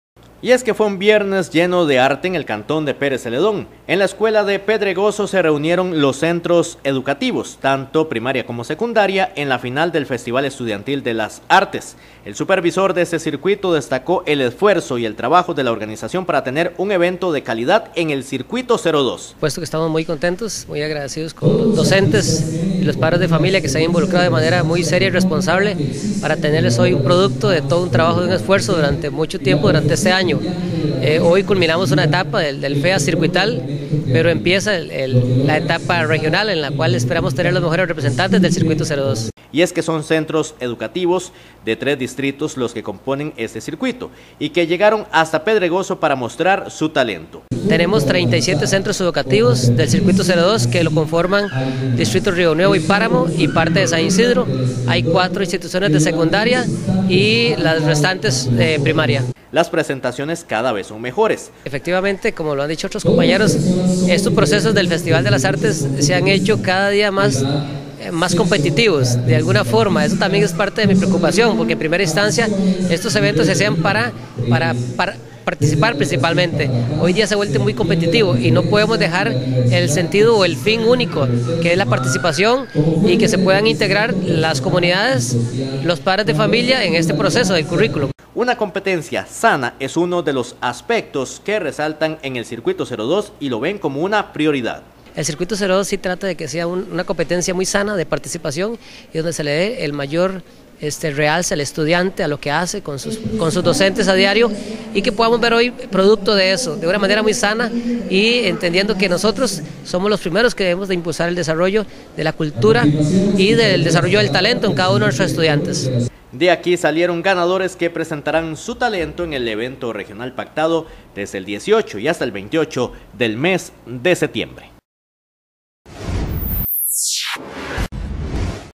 Escuela de Pedregoso vivió una gran fiesta con la final del FEA del circuito 02
Fue un viernes lleno de arte en el cantón de Pérez Zeledón, en la Escuela de Pedregoso se reunieron los centros educativos, tanto primaria como secundaria en la final del Festival Estudiantil de las Artes del circuito 02.